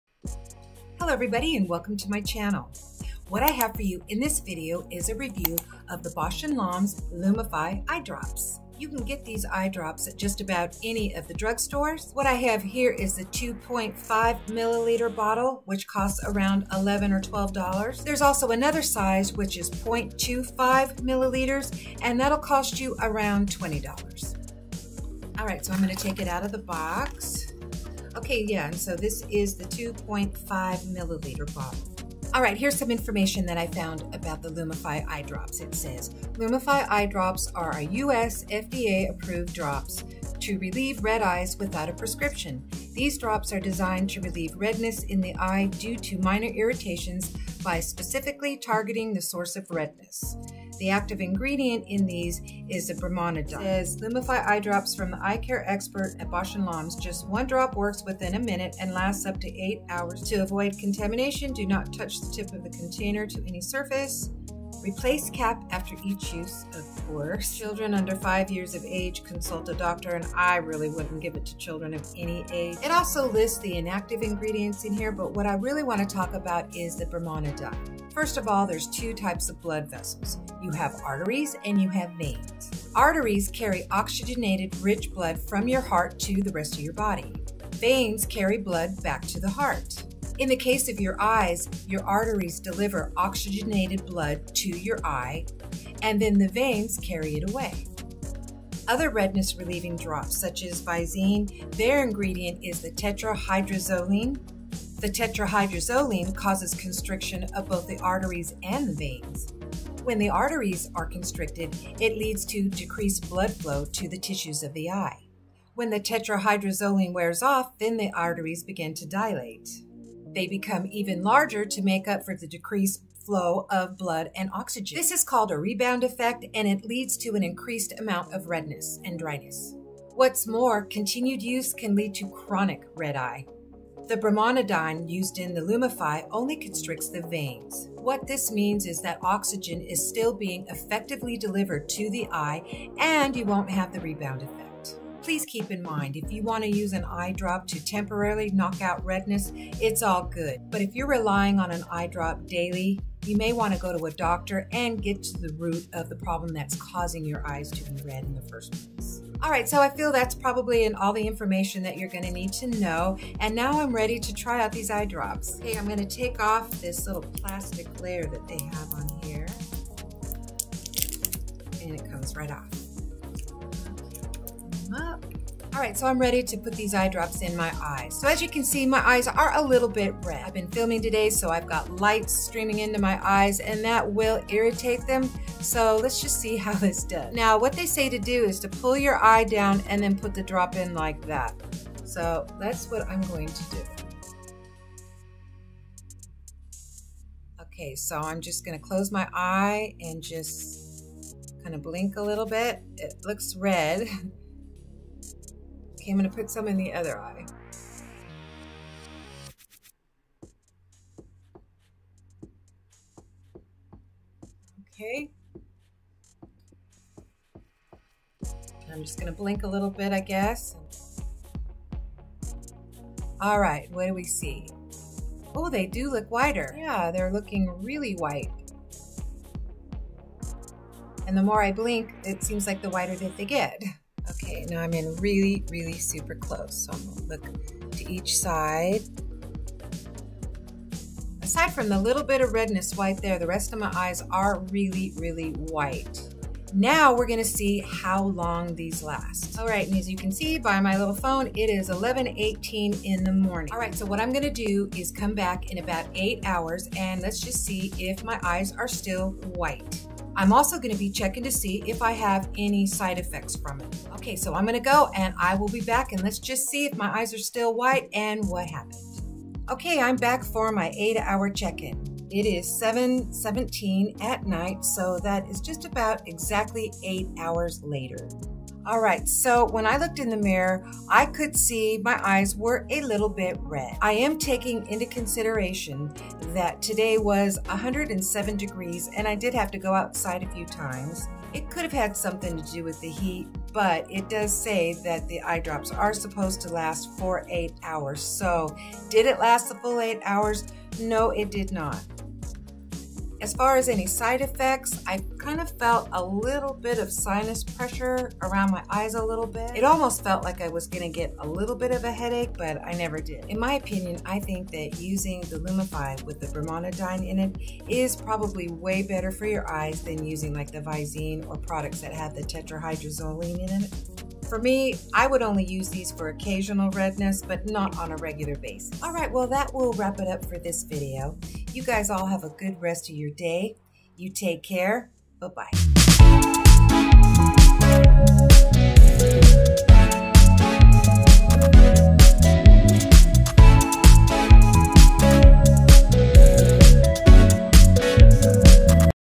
Lumify Eyedrops/Do They Really Last 8 HRS?? Review/Demo/Info!
In this video I give the Lumify eyedrops the 8 hour test to see if their claims of lasting for 8 hours are true!   I give you a full demo as well as information about how they work and the difference between  Bausch + Lomb Lumify VS Visine redness remover eyedrops!    Let's get connected On Facebook/Instagram/Twitter!